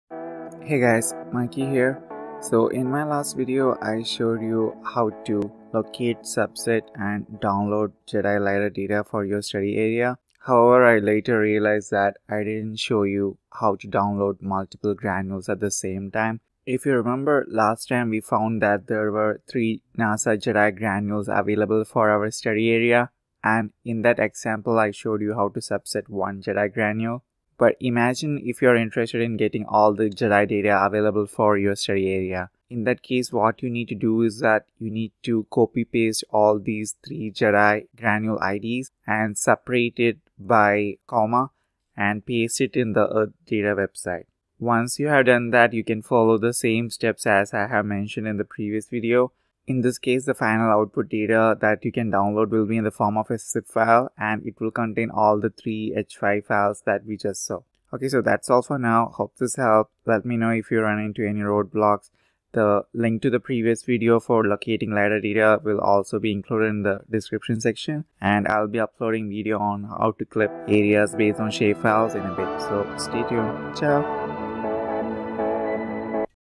Downloading multiple NASA GEDI LiDAR Granules simultaneously (Lidar remote sensing | Part 7)
In the previous videos, we talked about the most recent and freely available spaceborne lidar data from the NASA Global Ecosystem Dynamic Investigation (GEDI) Mission and saw how to locate lidar data for your AOI. In this short video, we will be looking at how to download multiple lidar granules of your study area in one go.